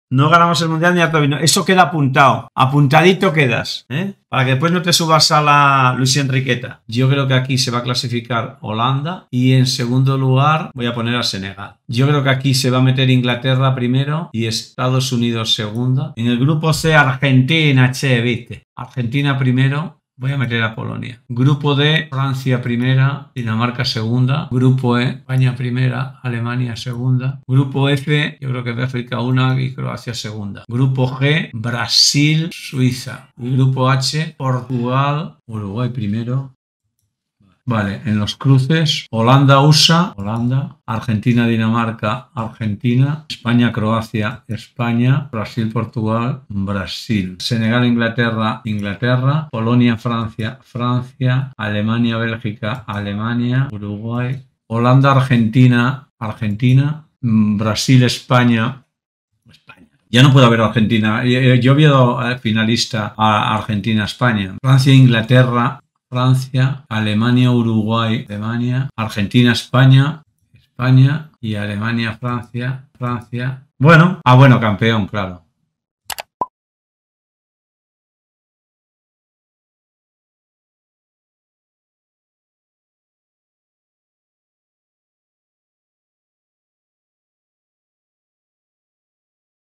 SÉ QUIÉN GANARÁ EL MUNDIAL DE QATAR 2022
Tras un gran análisis sobre todos los equipos, te digo mi predicción sobre la Copa del Mundo de Qatar 2022

✍️ Tus comentarios también son importantes, así que no dudes en dejar tu opinión